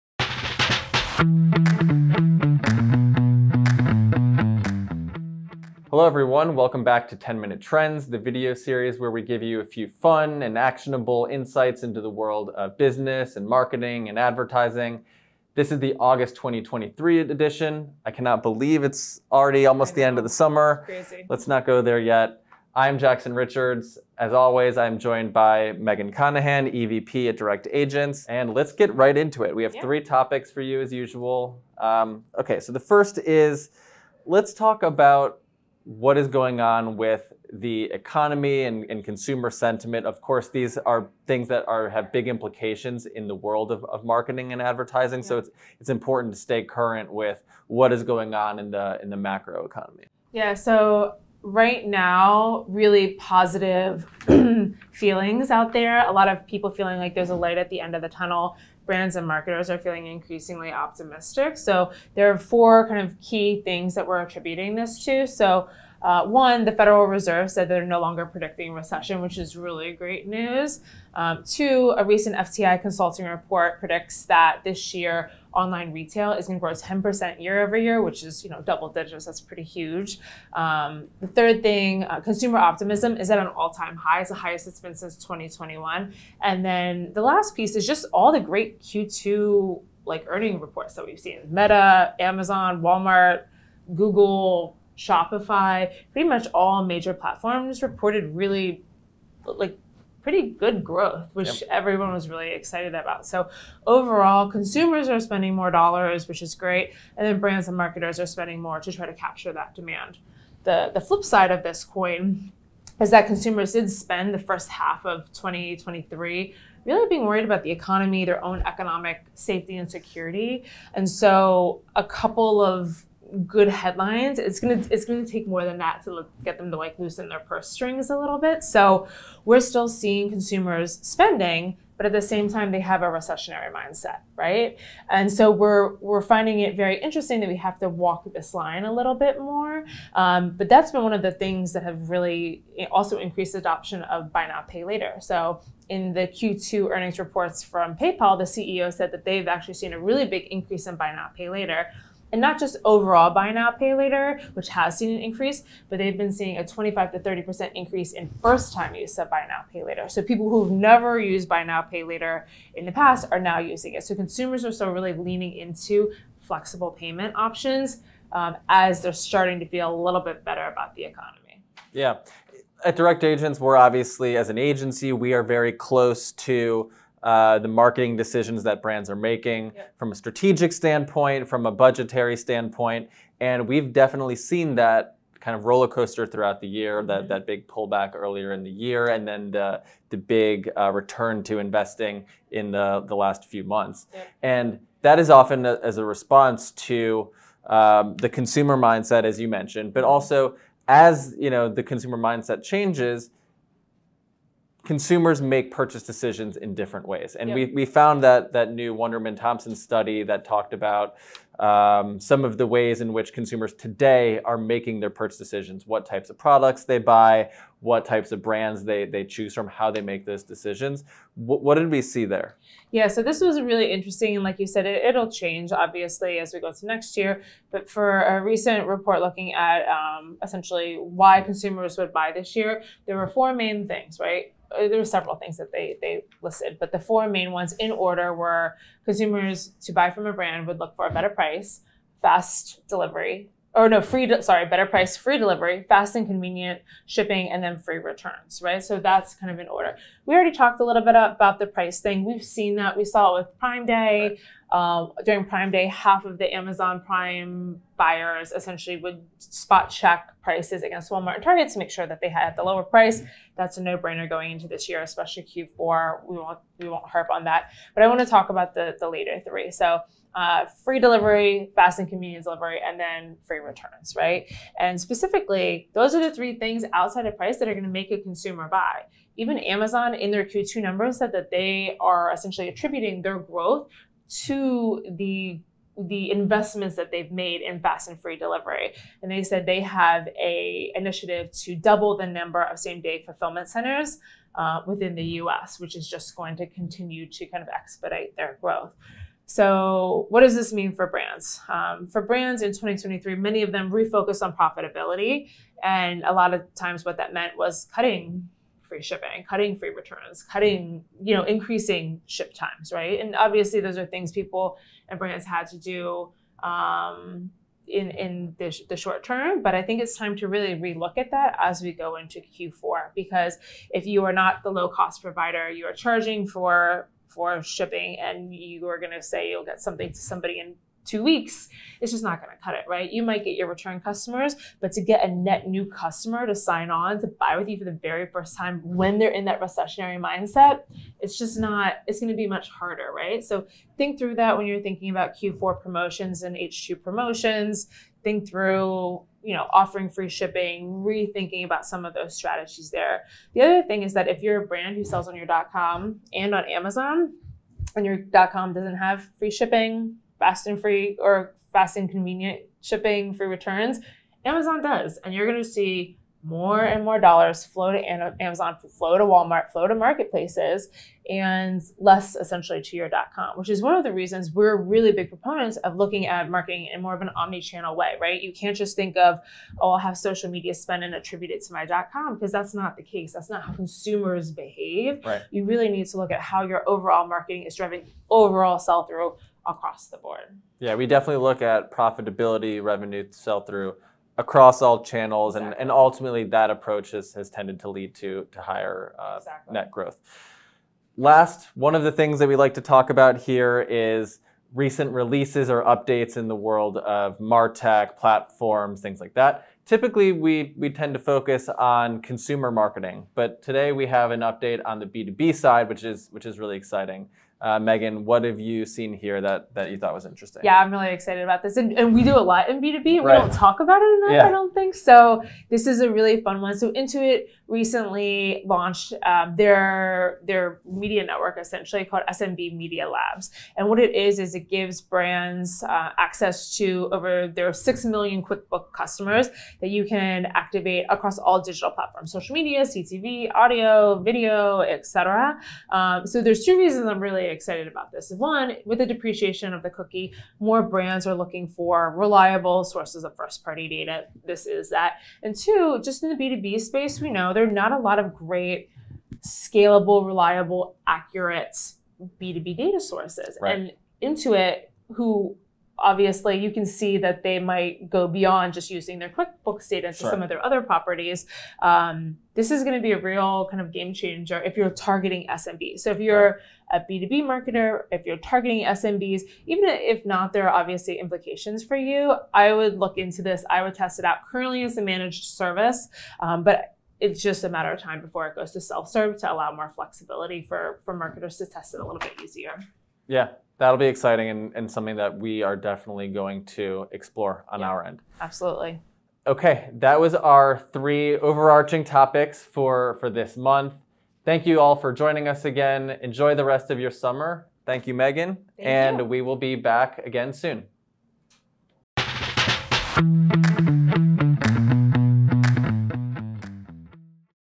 Direct Agents 10 Minute Trends 2023: Episode 4
Explore the latest economic trends with Jackson Richards and Megan Conahan, from positive market indicators to the rise of Buy Now Pay Later, and discover the key factors influencing consumers' buying decisions in Q4.

Direct Agents is an independent, full-service digital marketing agency specializing in performance-driven customer acquisition solutions.